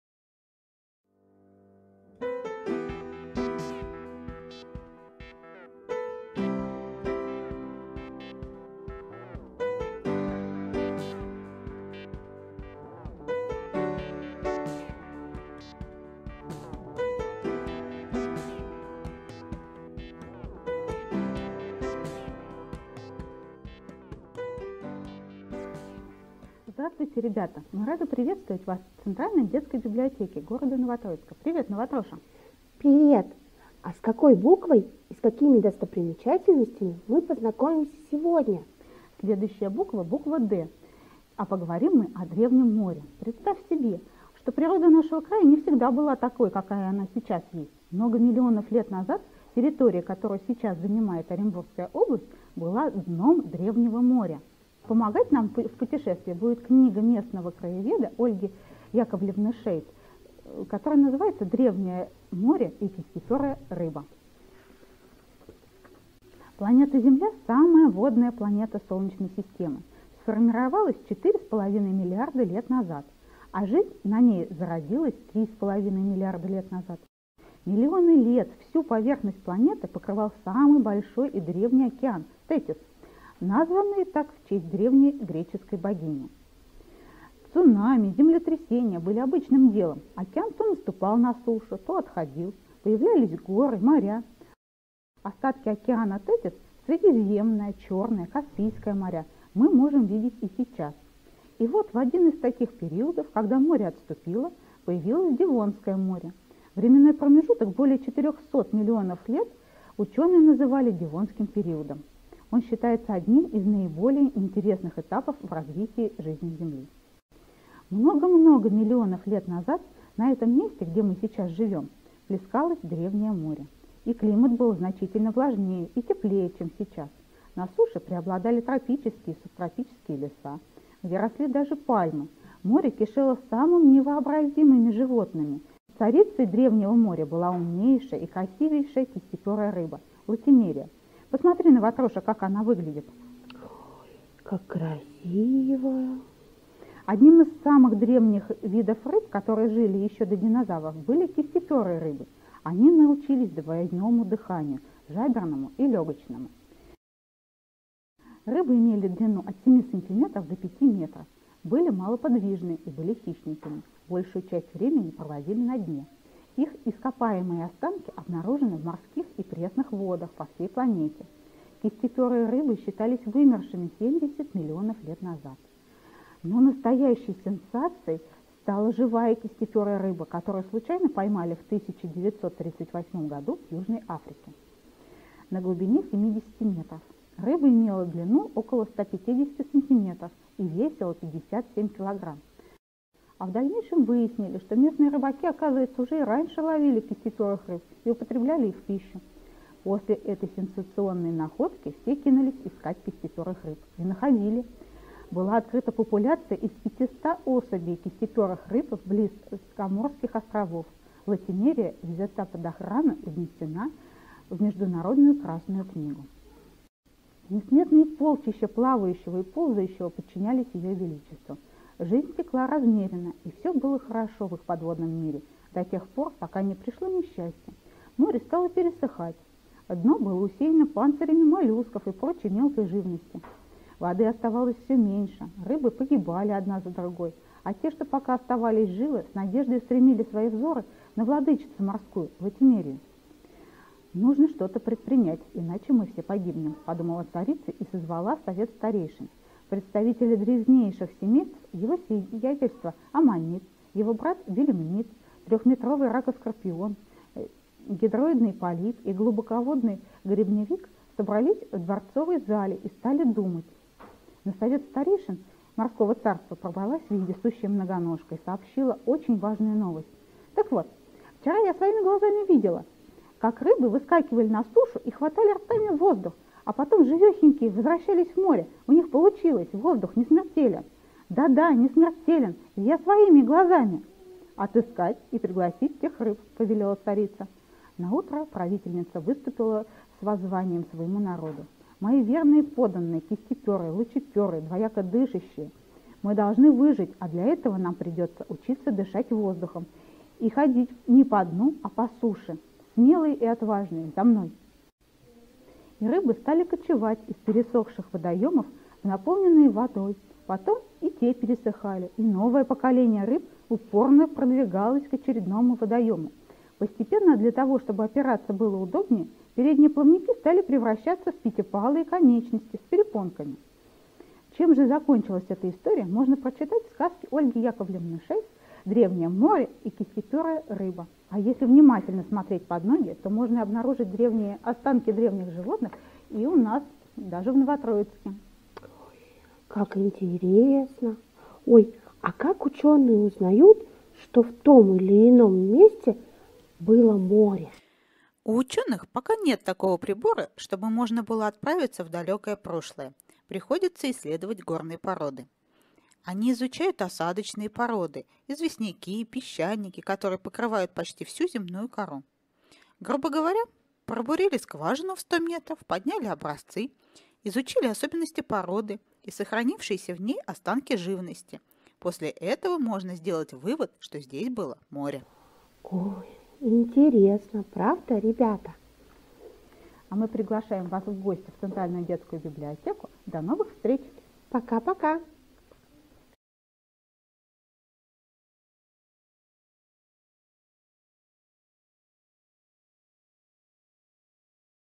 Выпуск 5 Азбука Оренбуржья Краеведение от А до Я (Д)
Приглашаем познакомиться со вторым выпуском образовательного проекта по краеведению Центральной детской библиотеки г. Новотроицка